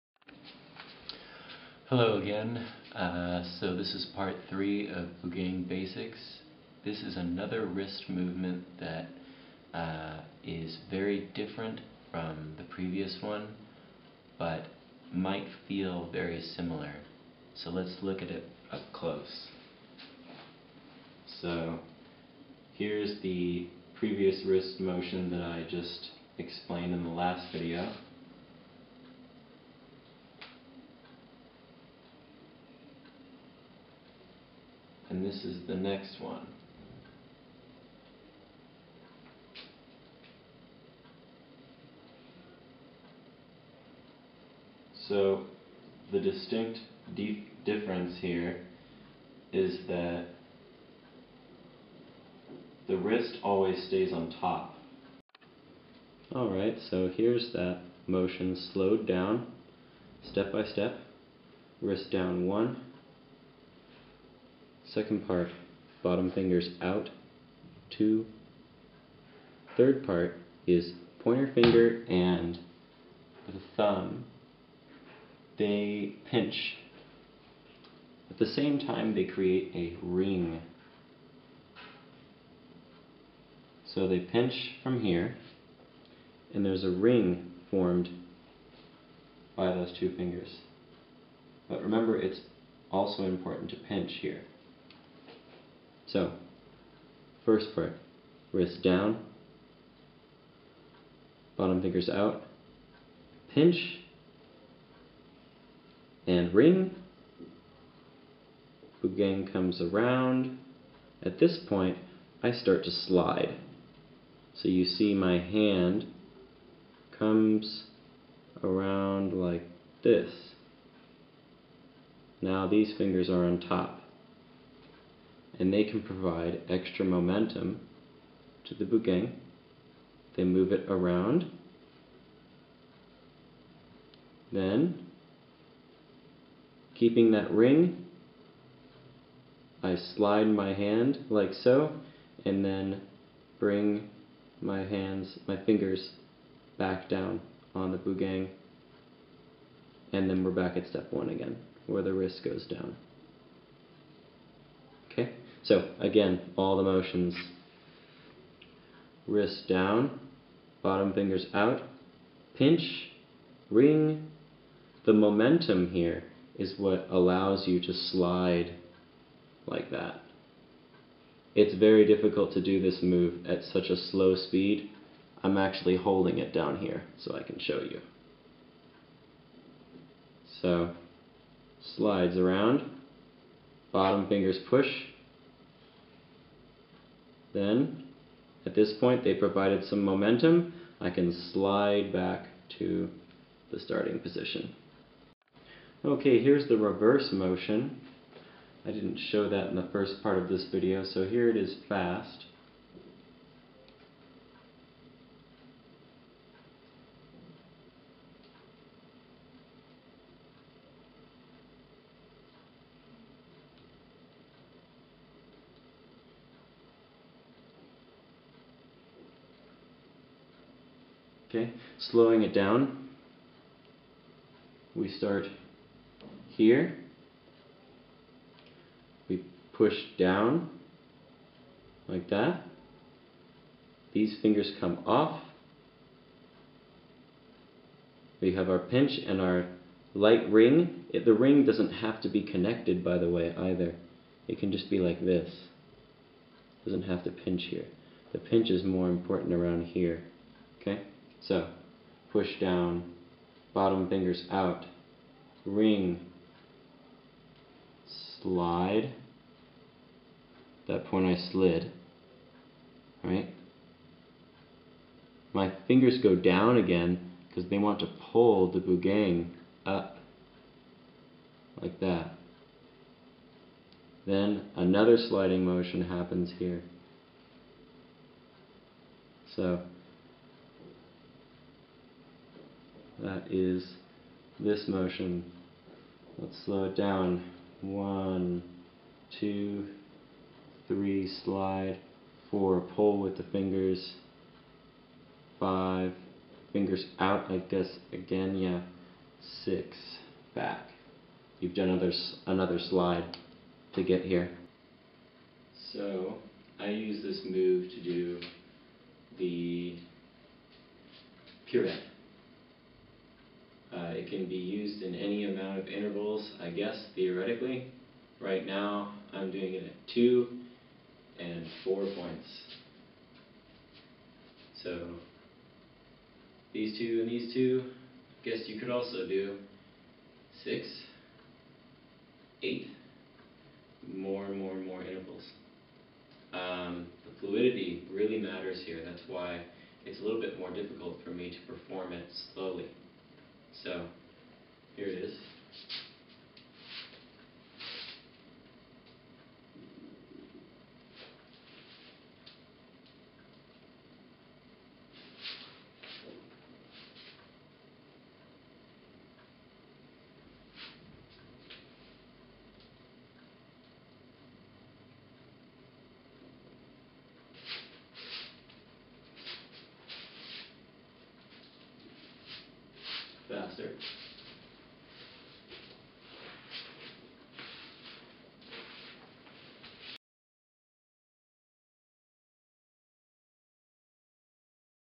Buugeng Basics III | Fundamental Motions | Wrist Movement II
My final movement I've been using for a little while now. If you would like to order Buugeng just like these or better, head to find which piece interests you.